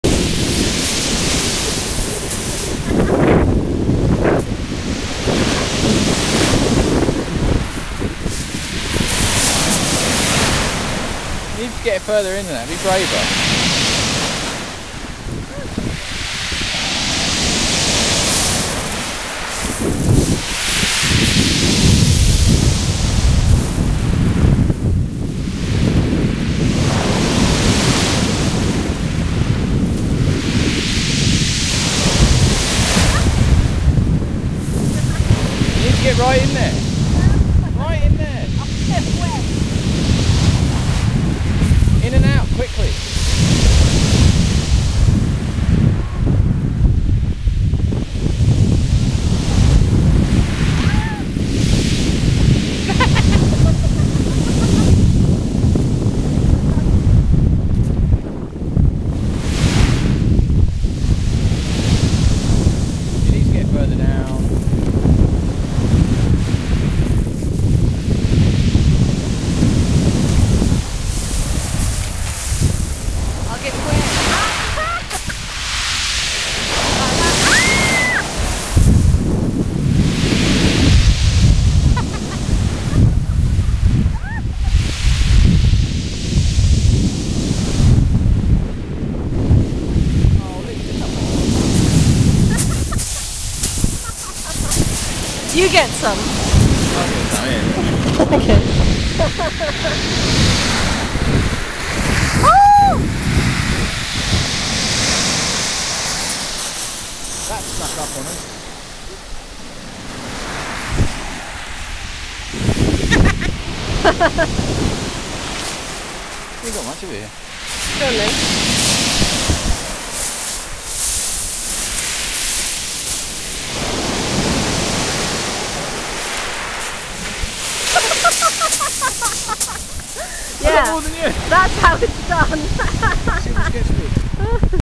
chesil beach
trying to gather water from the sea at chesil beach for my collection.
(the shingle is a lot steeper than it looks, and the sea is very cold!)